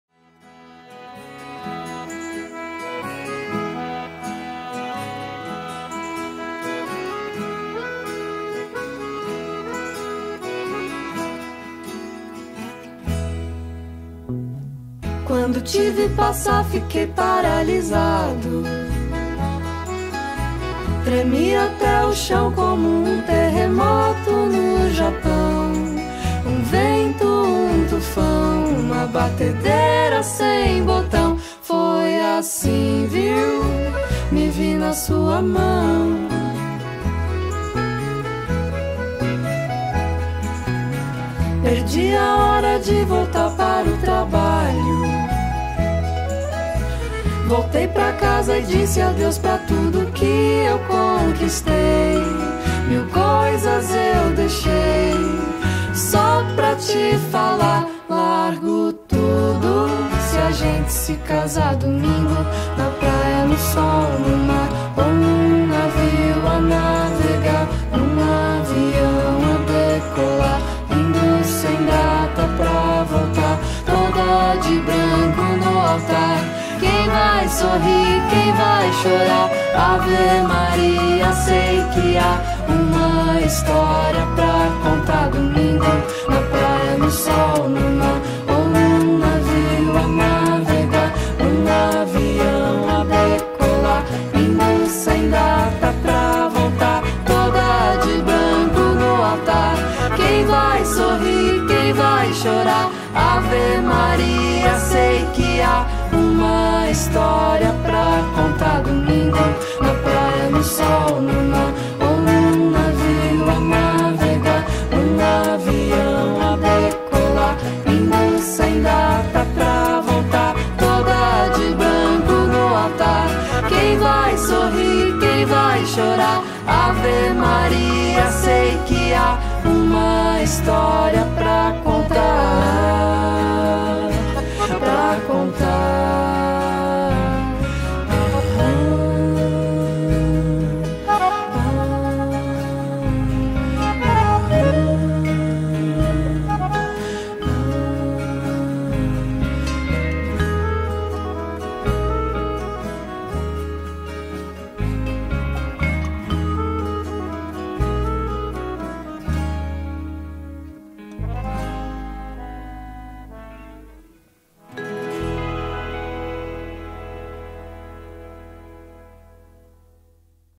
Beijos, abraços e sorrisos...
Imagens do casamento de Gabriely e Idelfonso.
Fotografia: Silvia Ribas
Filmagem e edição: Noitibus Estúdio